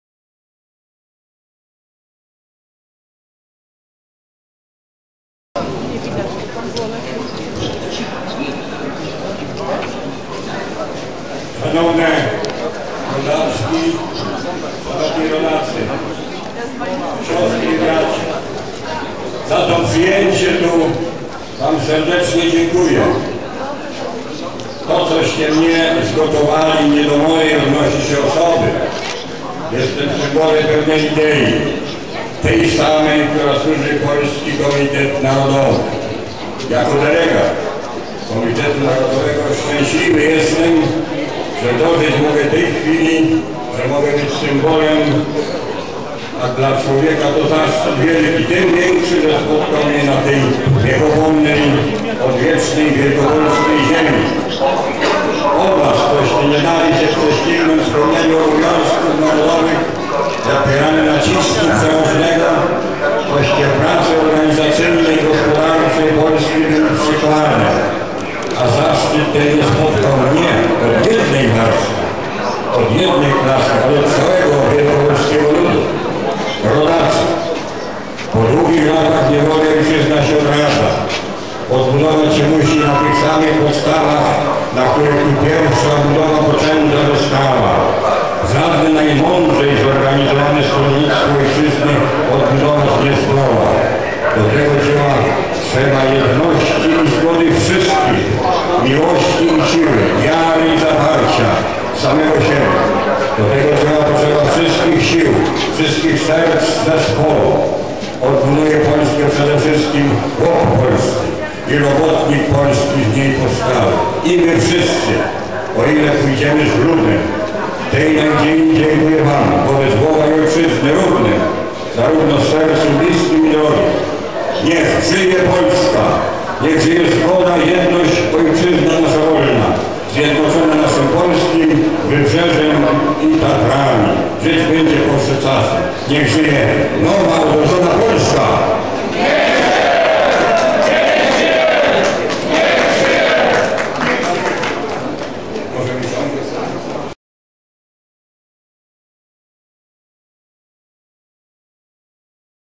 Paderewski w Poznaniu
Przemówienie Ignacego Paderewskiego do Mieszkańców Poznania